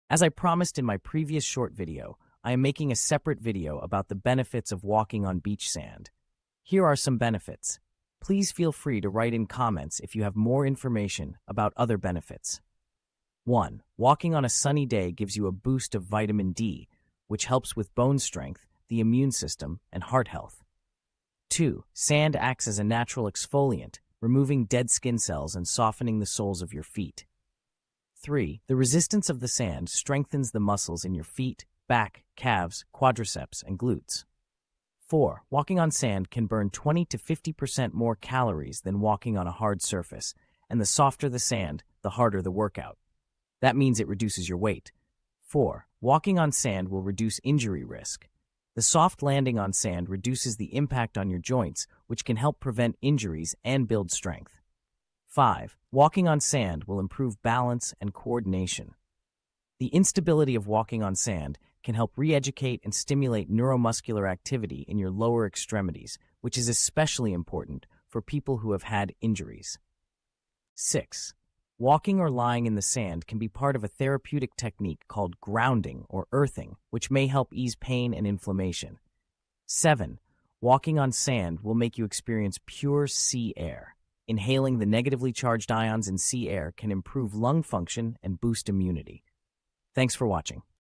Benefits of walking on the beach sand...
Copyright content of Naturally Engaging Motivator Channel.

As I promised in my previous short video, I am making a separate video about the benefits of walking on beach sand. Here are some benefits. Please feel free to write in comments if you have more information about other benefits.
1) Walking on a sunny day gives you a boost of vitamin D, which helps with bone strength, the immune system, and heart health.
2) Sand acts as a natural exfoliant, removing dead skin cells and softening the soles of your feet.
3) The resistance of the sand strengthens the muscles in your feet, back, calves, quadriceps, and glutes.
4) Walking on sand can burn 20–50% more calories than walking on a hard surface, and the softer the sand, the harder the workout. That means it reduces your weight
4) Walking on sand will reduce injury risk. The soft landing on sand reduces the impact on your joints, which can help prevent injuries and build strength.
5) Walking on sand will improve balance and coordination. The instability of walking on sand can help re-educate and stimulate neuromuscular activity in your lower extremities, which is especially important for people who have had injuries.
6) Walking or lying in the sand can be part of a therapeutic technique called "grounding" or "earthing", which may help ease pain and inflammation.
7) Walking on sand will make you experience pure sea air. Inhaling the negatively charged ions in sea air can improve lung function and boost immunity.